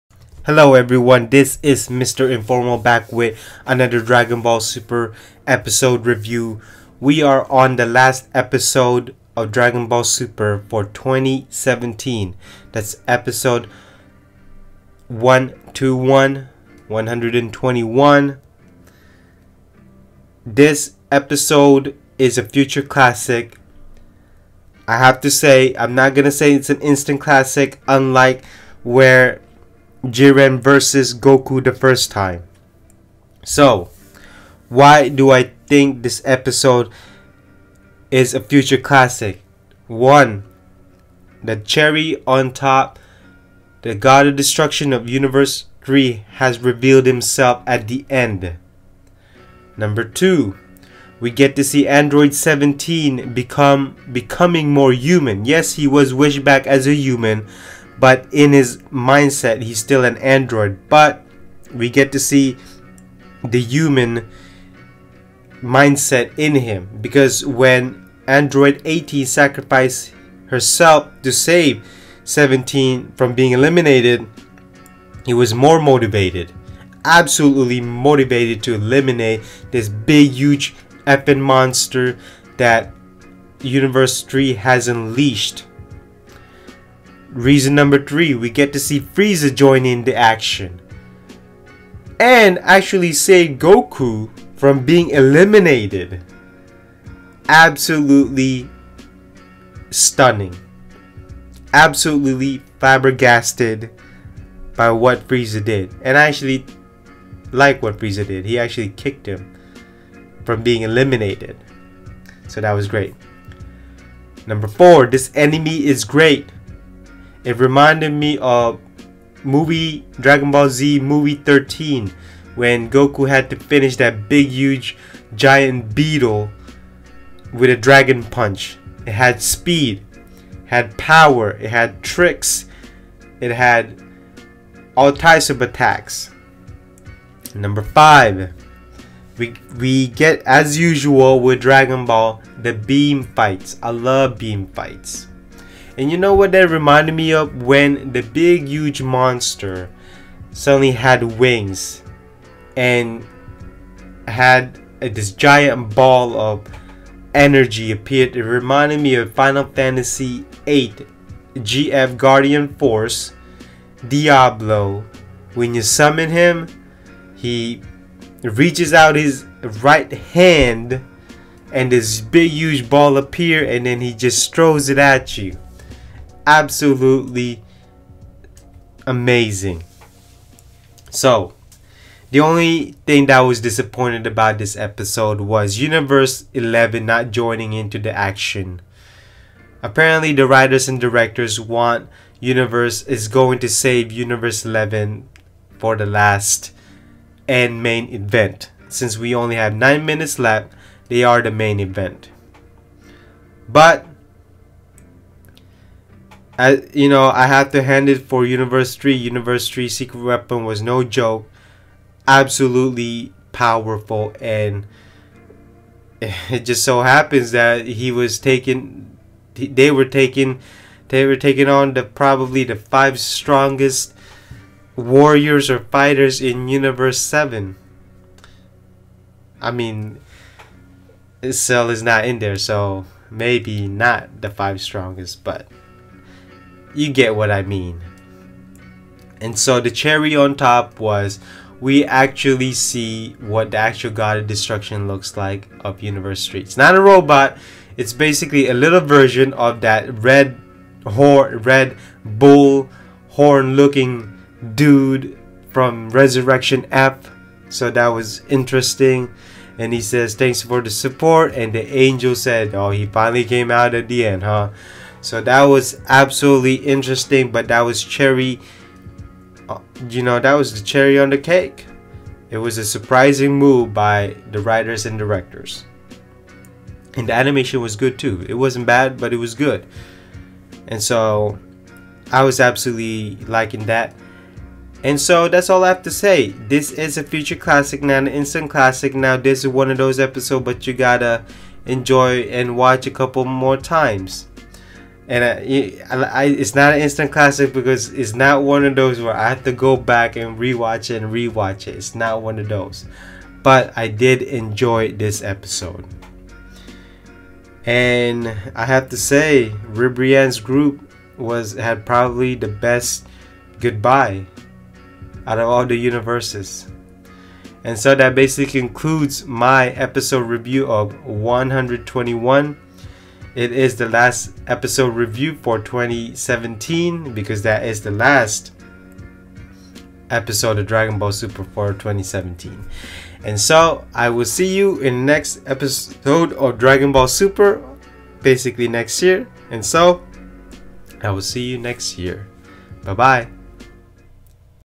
Dragon Ball Super 121 Review
Universe 3 has revealed their secret weapon and it is big. Can Universe 7 Eliminate them? My review of episode 121 of Dragon Ball Super